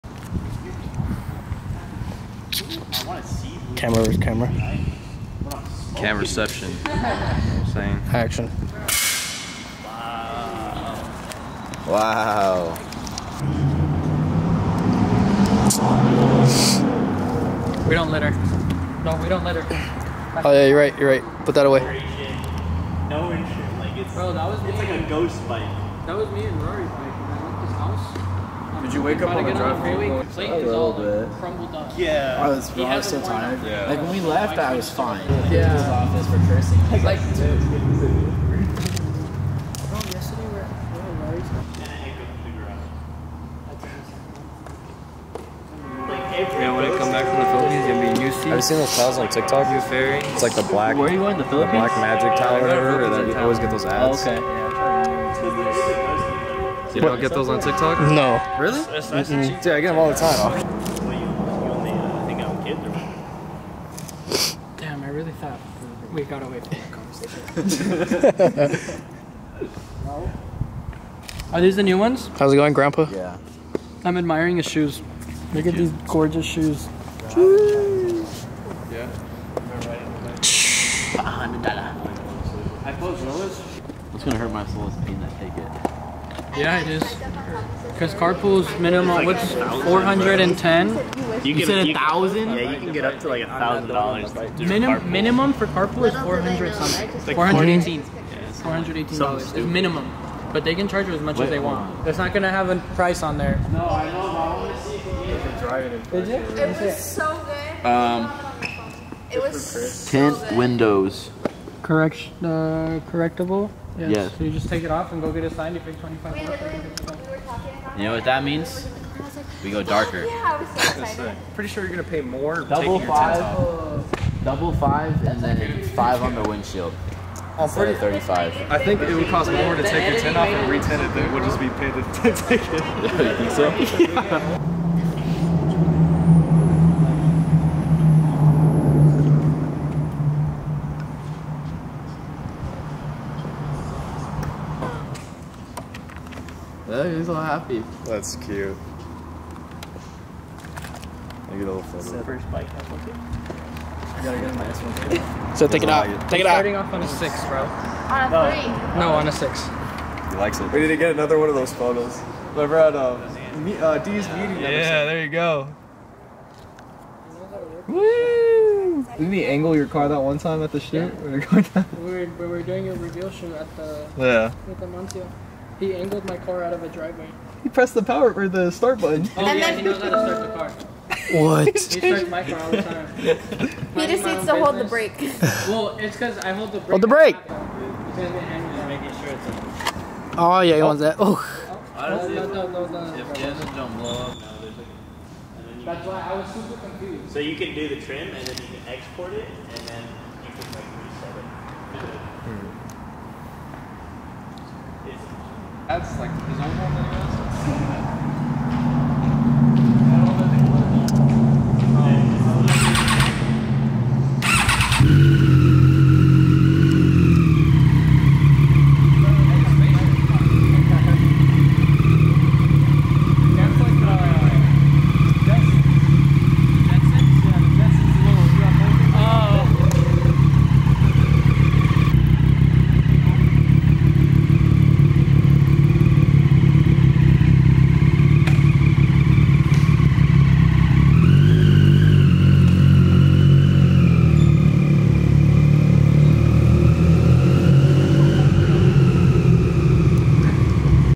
Clutch Supply Vlog 1 - BTS
Behind the scenes of one of our product shoots.

clutch supply,clutch,behind the scenes,clutch supply la,cars,car photoshoot,behind the scenes of a photoshoot,motorcycle photoshoot,motorcycle